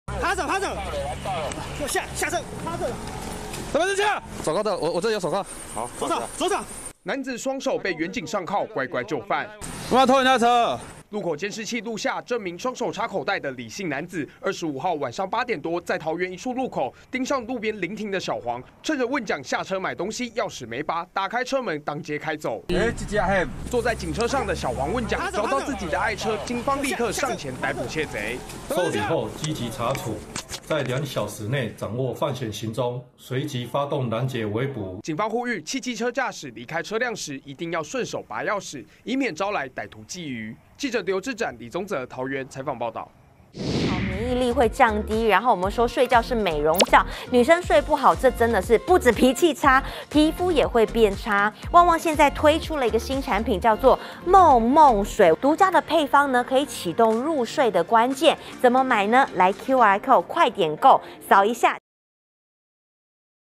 【每日必看】運將臨停未拔鑰匙 5秒被開走 逛大街2hrs落網 20230226
汽機車駕駛下車前，一定要"記得拔鑰匙"！桃園一名男子趁著小黃運將下車買東西，沒有拔走鑰匙，竟然就順手開走了計程車。在市區開車閒晃不到2小時，遭到警方逮捕。